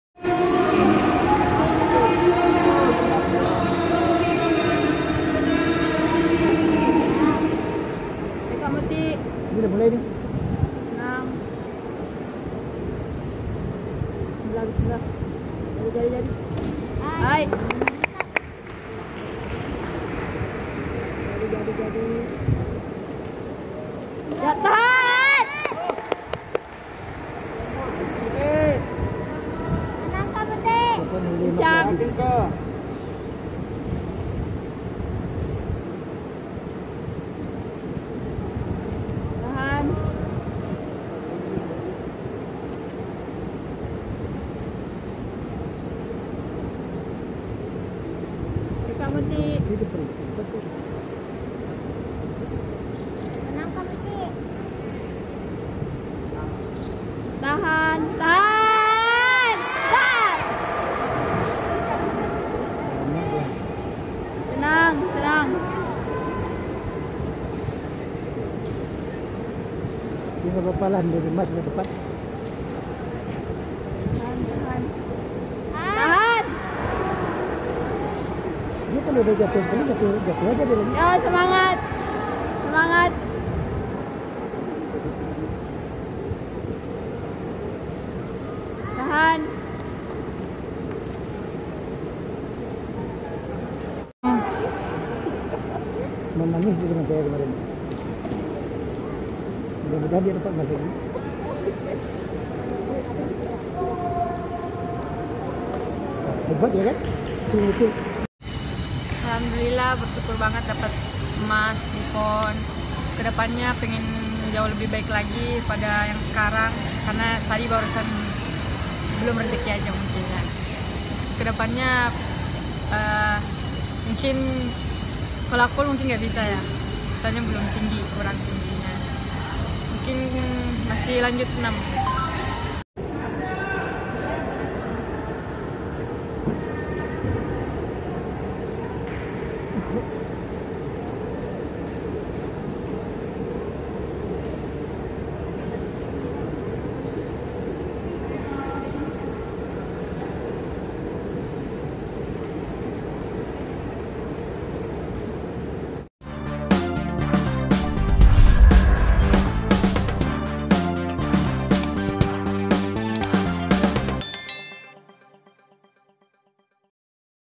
Aksi Mutiah Nur Cahya, Atlet Senam Artistik di PON Papua, Sumbang Emas bagi Sulsel
Peraih emas kedua Sulsel, Mutiah Nur Cahya Senam Artistik (alat palang bertingkat).

Mutiah meraih medali emas setelah meraih poin 11.033.

Mengalahkan tiga rekan pelatnasnya yakni, Tasza Miranda dari Jatim yang hanya mengumpulkan 11.000,

serta dua wakil DKI Jakarta, Badia Indah 10.500 dan

Sri Martiani dengan poin 10.167 di Gedung Senam Stadion Lukas Enembe. (sal)